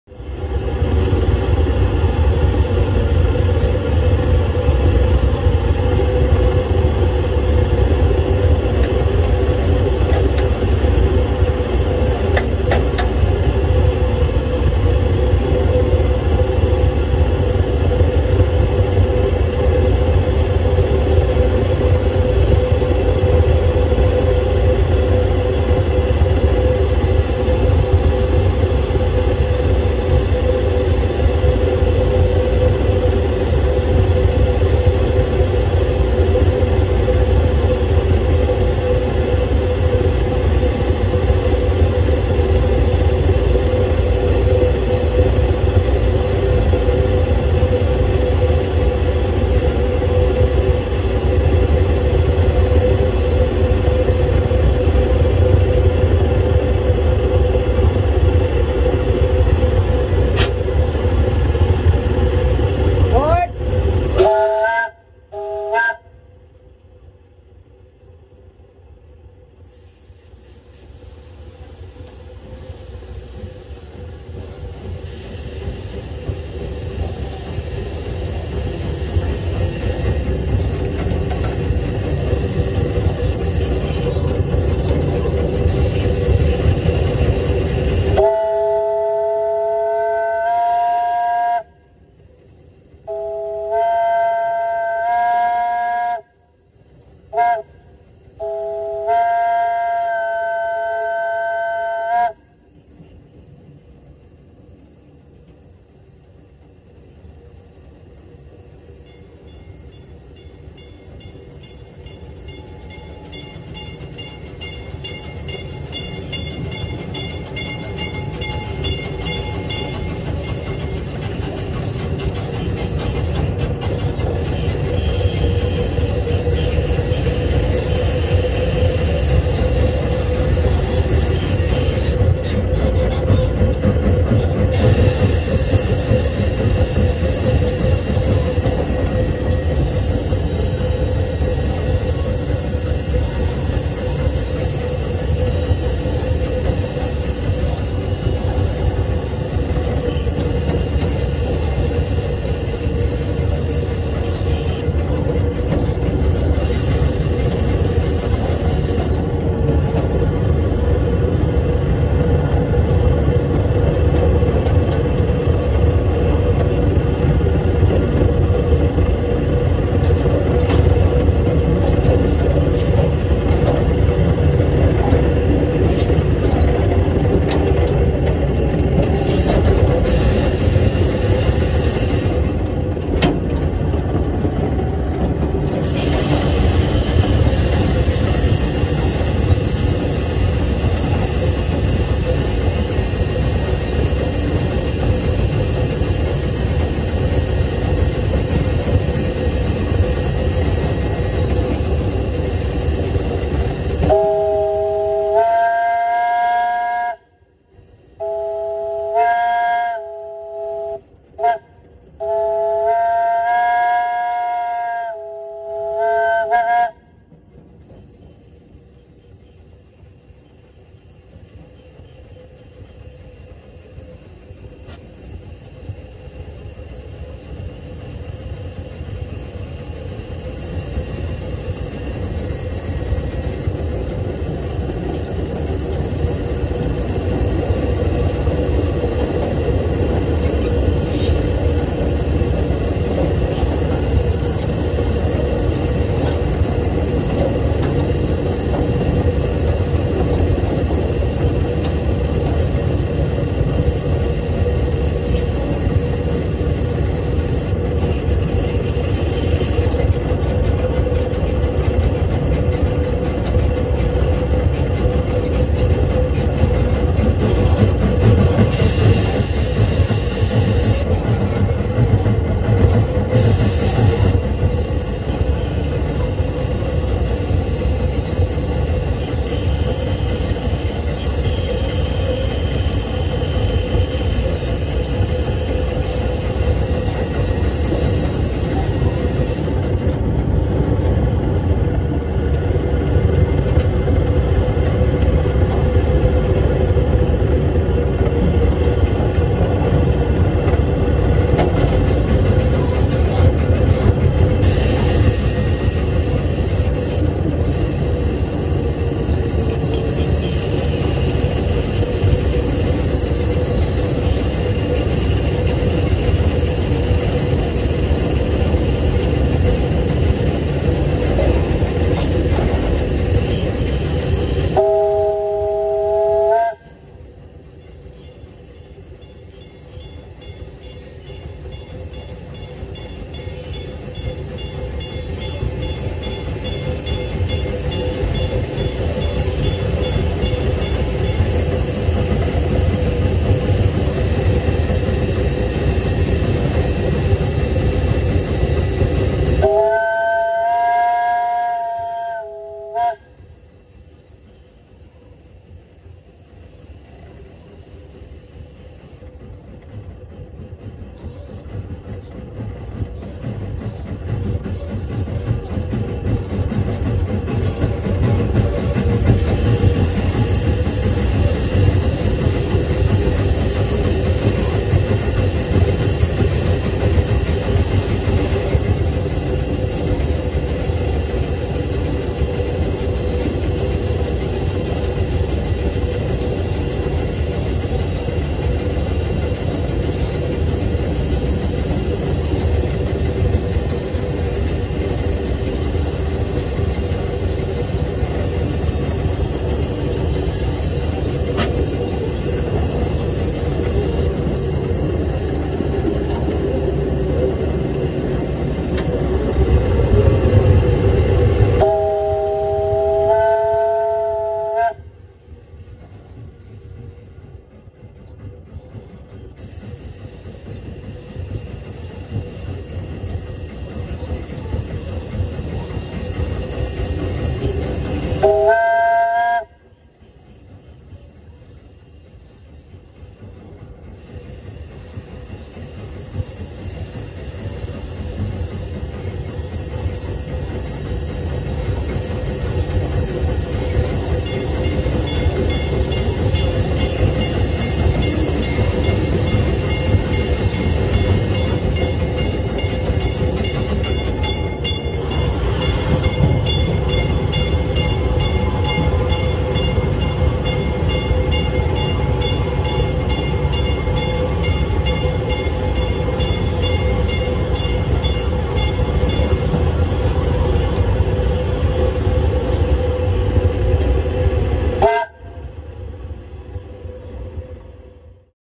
S&S Operational Experience
Here is a video of me operating Shelby and Southern No. 3 on the Shelby and Southern Steam Railroad at the Heart of Dixie Railroad Museum. No. 3  is a 24 inch gauge propane fired steam locomotive built by Crown Metal Products in 1975. The locomotive and it’s accompanying cars were built for and operated at the Birmingham Zoo from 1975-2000. It was acquired by the Heart of Dixie Railroad Museum in 2001 and ran regularly there up until 2012. About 9 months ago, the locomotive began undergoing an overhaul and is back and operational again for the first time in in nearly a decade.